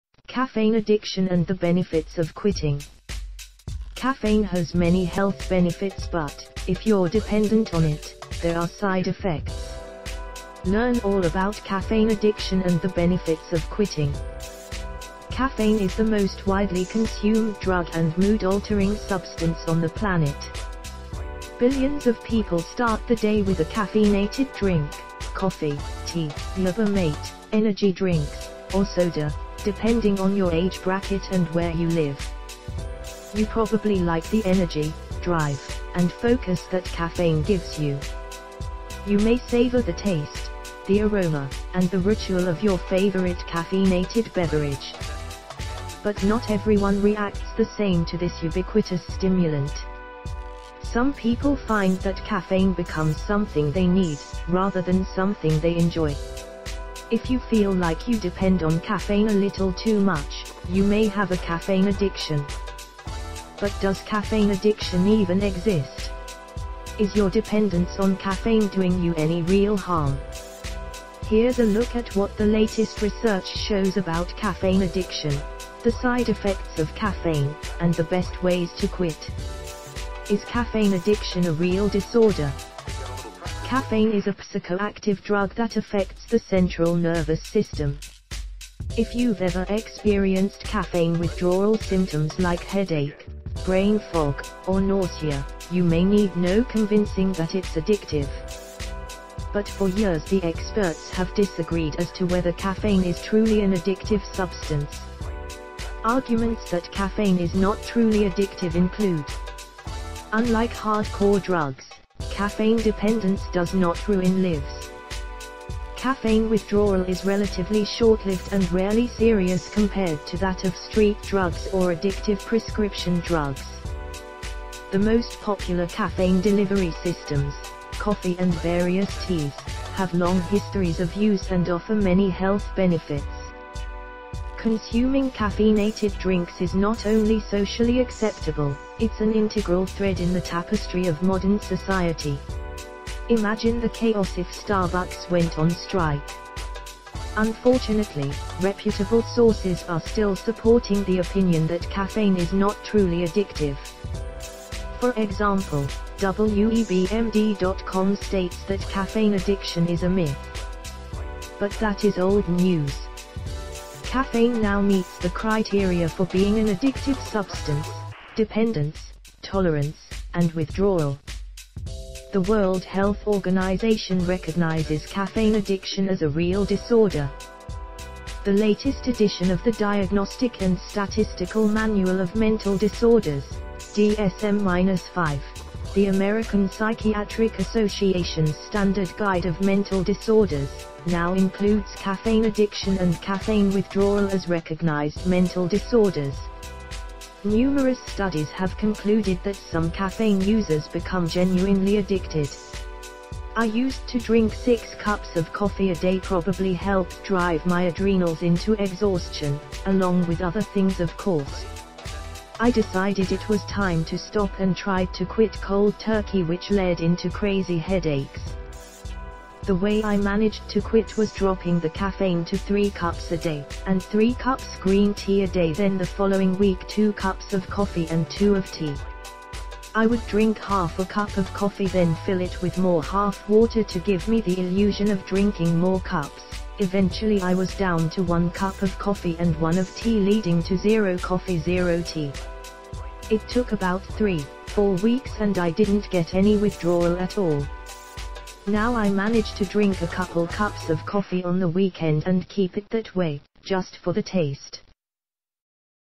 Caffeine Addiction and the Benefits of Quitting Coffee
Caffeine has many health benefits but, if you’re dependent on it, there are side effects. Learn all about caffeine addiction and the benefits of quitting.

Caffeine is the most widely consumed drug and mood-altering substance on the planet.

Billions of people start the day with a caffeinated drink — coffee, tea, yerba mate, energy drinks, or soda — depending on your age bracket and where you live.

You probably like the energy, drive, and focus that caffeine gives you.

You may savor the taste, the aroma, and the ritual of your favorite caffeinated beverage.

But not everyone reacts the same to this ubiquitous stimulant.

Some people find that caffeine becomes something they need, rather than something they enjoy.

If you feel like you depend on caffeine a little too much, you may have a caffeine addiction.

But does caffeine “addiction” even exist?

Is your dependence on caffeine doing you any real harm?

Here’s a look at what the latest research shows about caffeine addiction, the side effects of caffeine, and the best ways to quit.
Is Caffeine Addiction a Real Disorder?

Caffeine is a psychoactive drug that affects the central nervous system.

If you’ve ever experienced caffeine withdrawal symptoms like headache, brain fog, or nausea, you may need no convincing that it’s addictive.

But for years the experts have disagreed as to whether caffeine is truly an addictive substance.

Arguments that caffeine is not truly addictive include:

Unlike hard-core drugs, caffeine dependence does not ruin lives.

Caffeine withdrawal is relatively short-lived and rarely serious compared to that of street drugs or addictive prescription drugs.

The most popular caffeine delivery systems — coffee and various teas — have long histories of use and offer many health benefits.

Consuming caffeinated drinks is not only socially acceptable, it’s an integral thread in the tapestry of modern society. Imagine the chaos if Starbucks went on strike!

Unfortunately, reputable sources are still supporting the opinion that caffeine is not truly addictive.

For example, WebMD.com states that caffeine addiction is a myth.

But that is old news.

Caffeine now meets the criteria for being an addictive substance: dependence, tolerance, and withdrawal.

The World Health Organization recognizes caffeine addiction as a real disorder.

The latest edition of the Diagnostic and Statistical Manual of Mental Disorders (DSM-5), the American Psychiatric Association’s standard guide of mental disorders, now includes caffeine addiction and caffeine withdrawal as recognized mental disorders.

Numerous studies have concluded that some caffeine users become genuinely addicted. You can find links to dozens of studies that support this in Caffeine Use Disorder: A Comprehensive Review and Research Agenda.